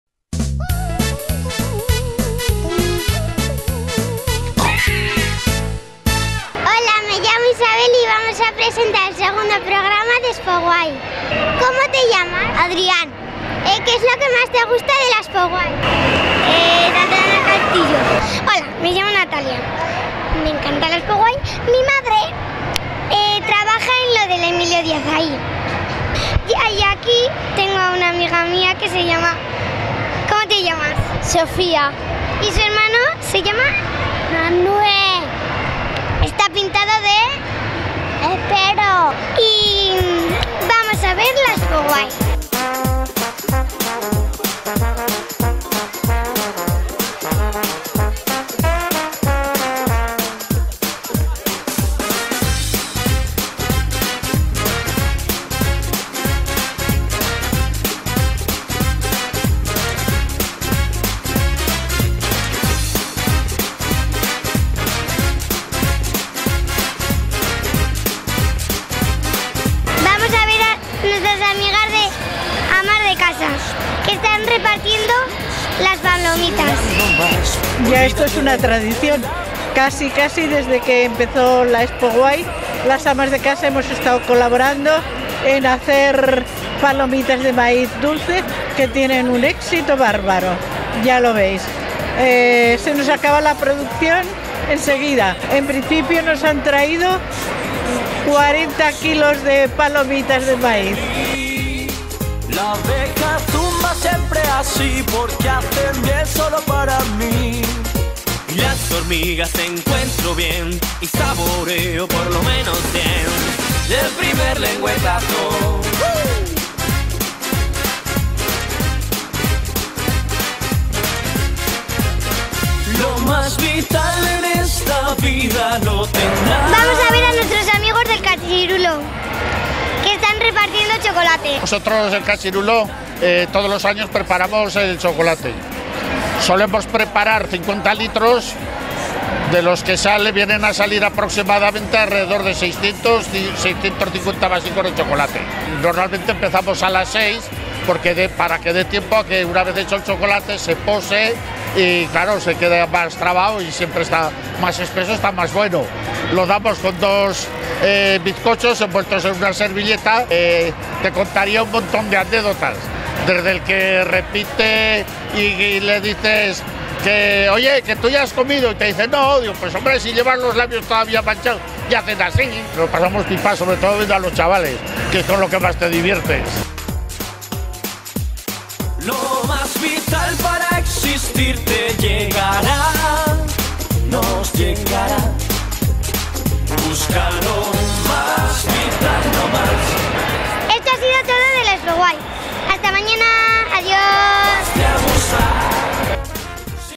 La Comarca.tv - Expoguay 2015, Lunes 28
Segundo día del mayor salón infantil del Bajo Aragón en Alcañiz, Expoguay. Las Amas de Casa y sus palomitas, el cachirulo con su Chocolate y cientos de niños llenaron de alegría y diversión el recinto de ferialcañiz.